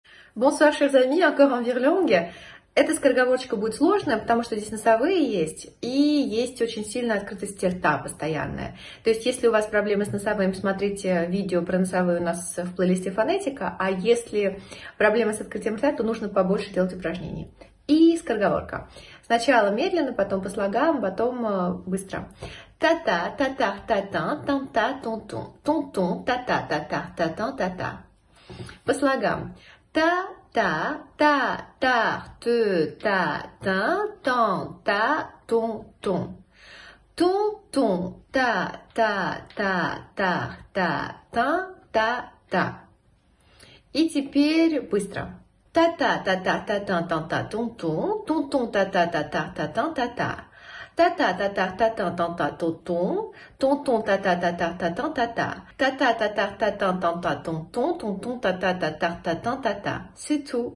Скороговорка на французском №16: Tata, ta tarte tatin tenta tonton.
👌🏻Тренировать произношение с нами — легко! А еще это прекрасная возможность посмеяться, ведь с первого раза ее произнести правильно практически невозможно)

Tata, ta tarte tatin tenta Tonton; Tonton tâta ta tarte tatin, Tata.

Читаем сначала медленно, потом по слогам, потом быстро 3 раза.

Попробуем перевести с Вами?)